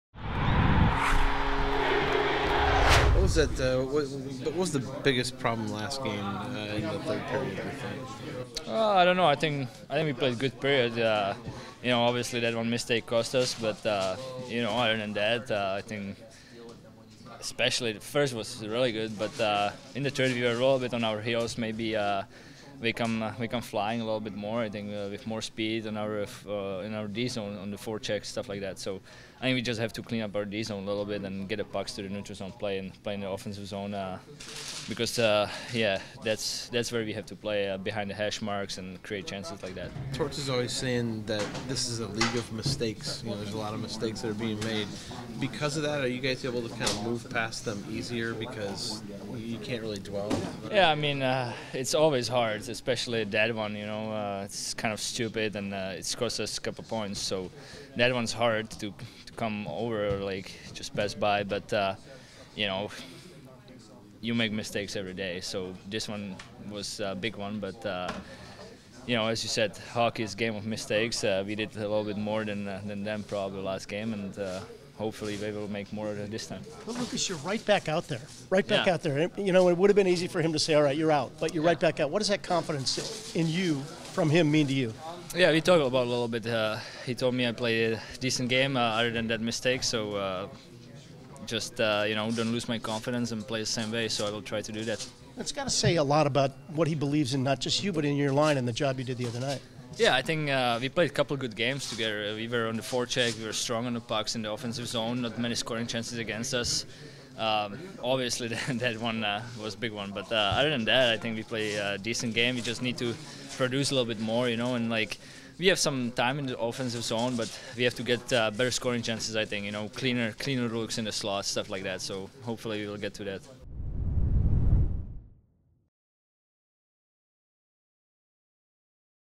In the Room: Lukas Sedlak 11/23/18
Columbus Blue Jackets forward Lukas Sedlak talks about cleaning up some mistakes from Monday and how he's looking to create more chances.